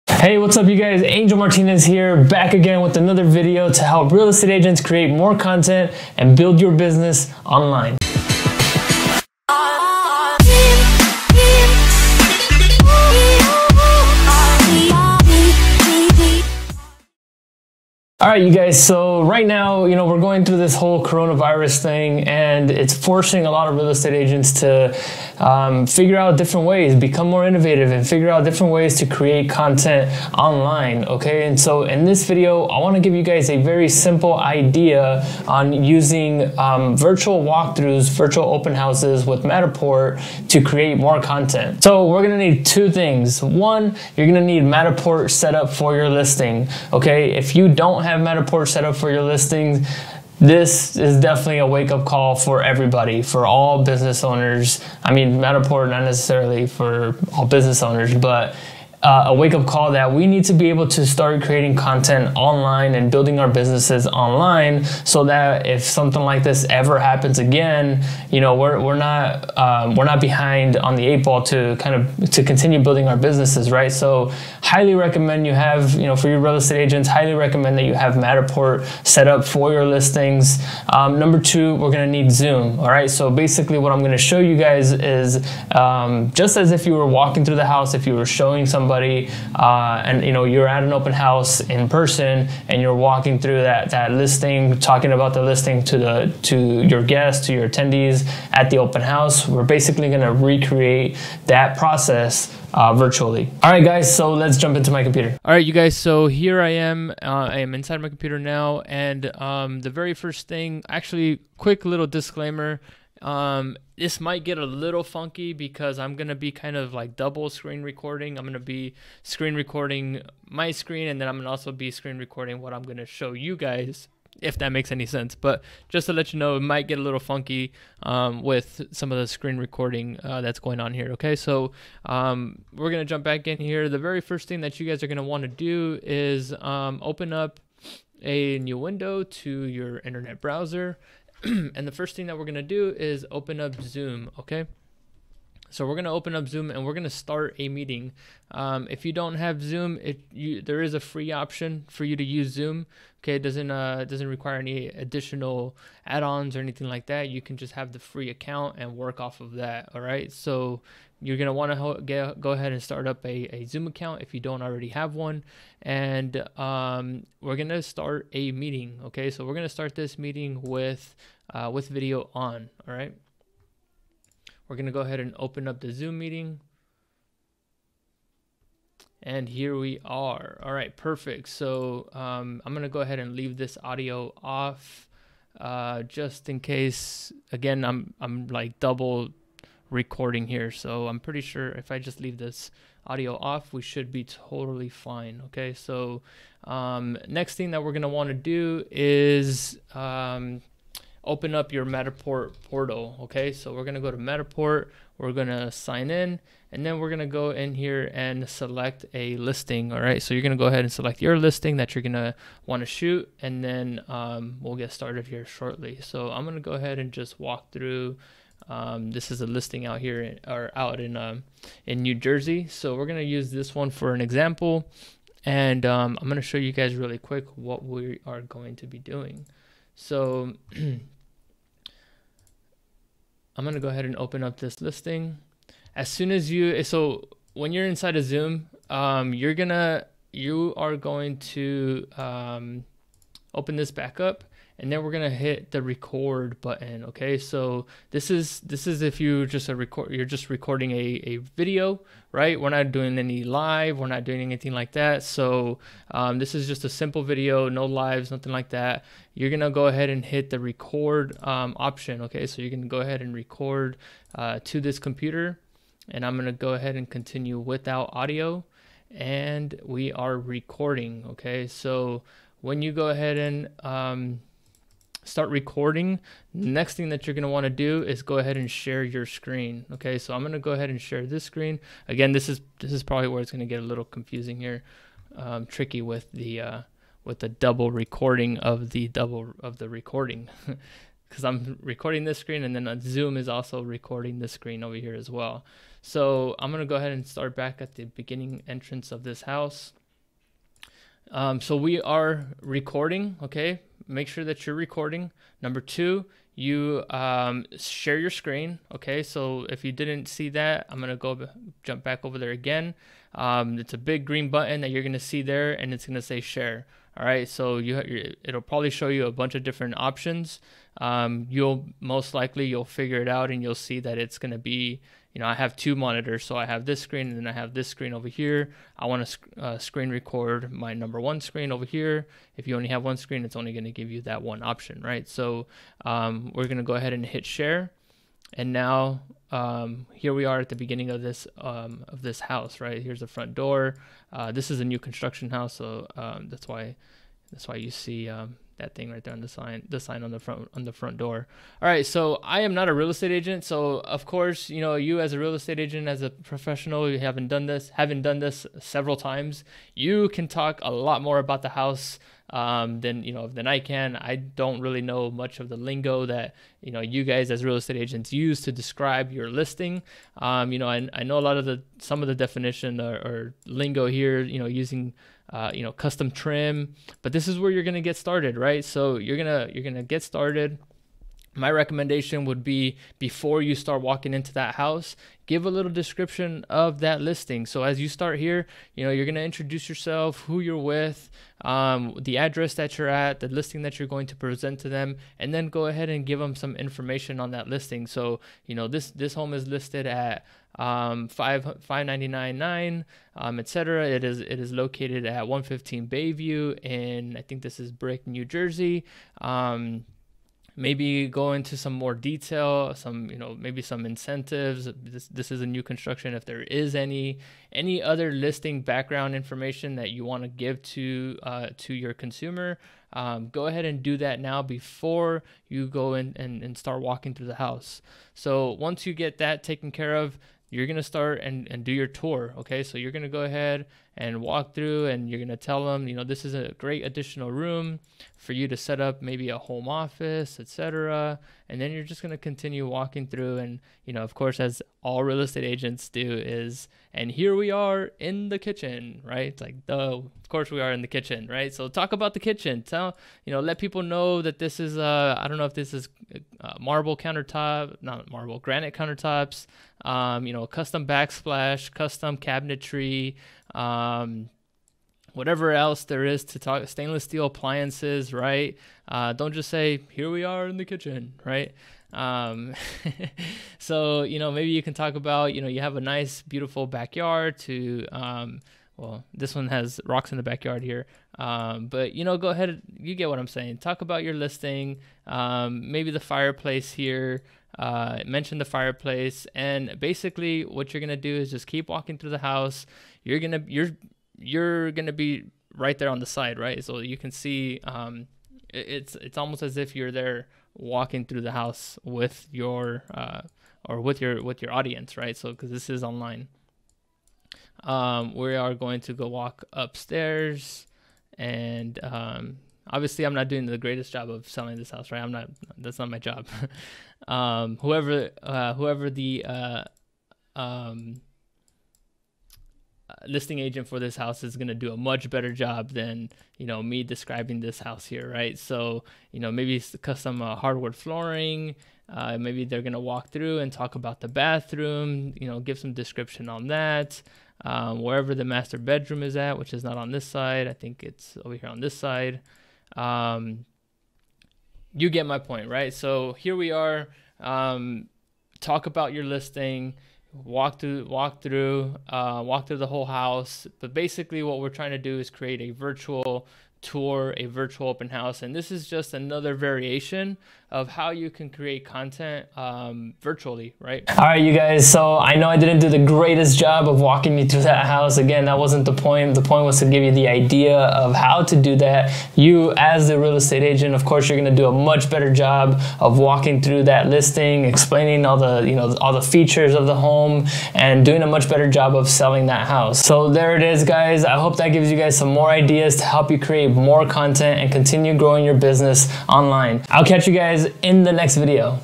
How to Create a Virtual Open House Tour Using Zoom and Matterport for Real Estate Agents
In this video, I give you a quick tutorial on how you can create a virtual open house using Zoom and Matterport. This is another way that you can adapt to the COVID 19 crisis and still continue to market and grow your business.

Use this to create content and bring value not only to your sellers but to your community. This is an opportunity to show your clients that you can and will adapt to any circumstance.

If you don't have a Zoom account you can create a FREE account to get the job done.